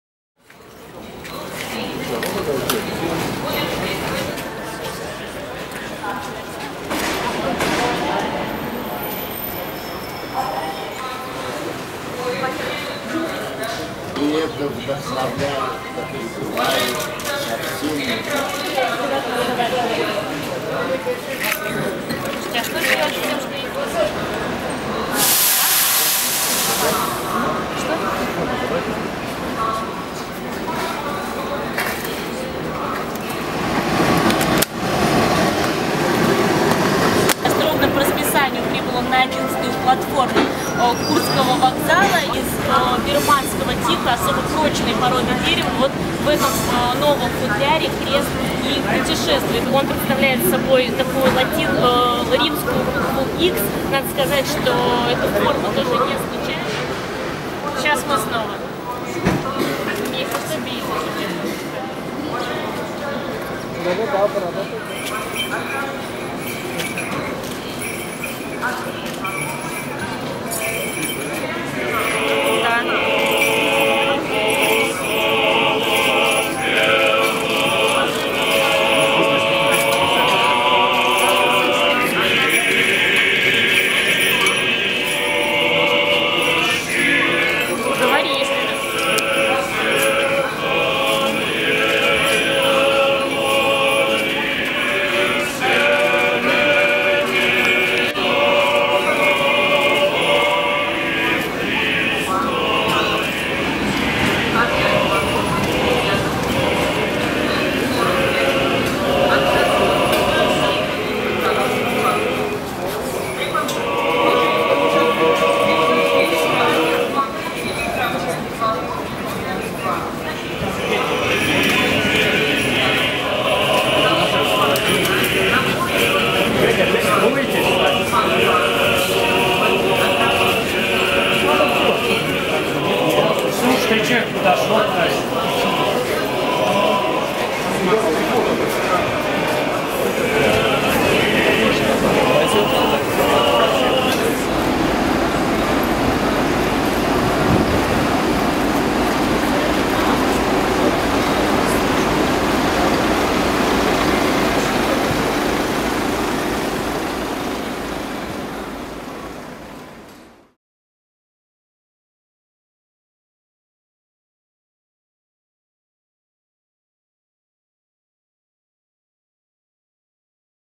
Спецпоезд и молебен на вокзале. В Москву доставили крест св. Андрея
В Москву из Петербурга привезли одну из самых почитаемых христианских святынь — крест апостола Андрея Первозванного. Поезд с реликвией на Курском вокзале встречал корреспондент АиФ.ru Роман Кульгускин. .